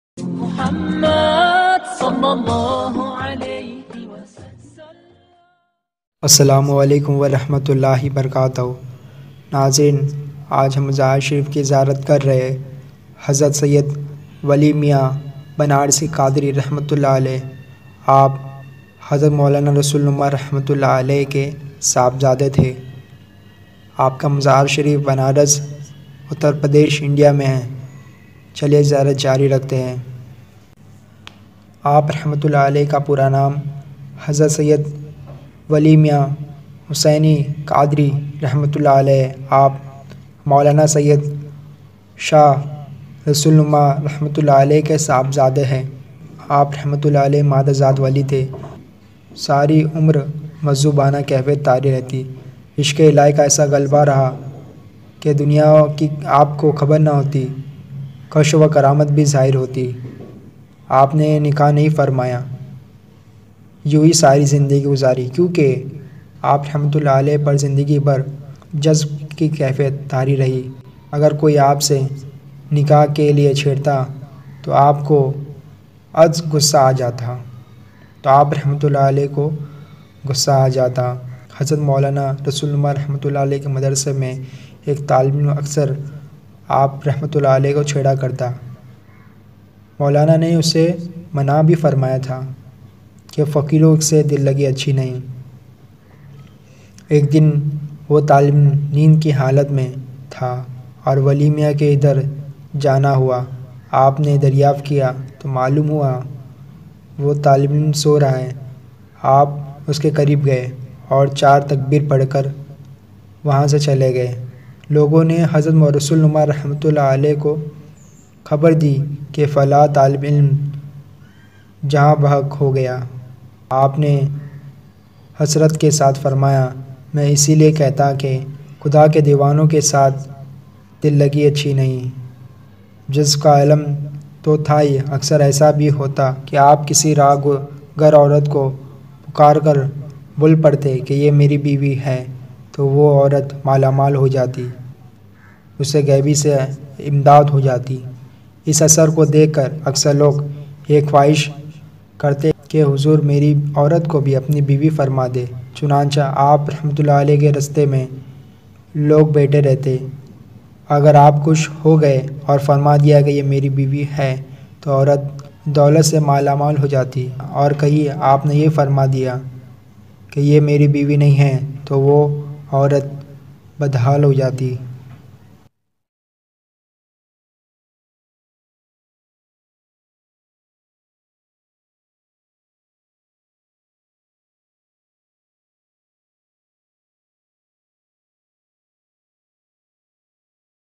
Aap Aurat Ko Biwi Kehte To Wo Paise Wali Ban Jati | WALI MIYA| औरत को अपनी बीवी कहा तो पैसे वाली बनी
GOOGLE MAP LOCATION:
Maulana Baba Dargah
Rabindranath Tagore Rd, Chittanpura, Aalampura, Varanasi, Uttar Pradesh, India

MAJZOOB WALI MIYA
HAZRAT SAYYED SHAH WALI MIYA HUSSAINI QADRI BANARASI (quddisa sirruhu)

URDU: حضرت مجذوب ولی میاں.

HINDI: मजजुब वली मिया

KNOWN FOR : Sufi Majzoob in Banaras.

RESTING PLACE : Maulana Baba Dargah, Banaras.

INFLUENCED BY : Hazrat Maulana Sayyed Rasool Numa Rehmatullah alaih.

Assalaumalaikum Friends,

JazakAllahKhair.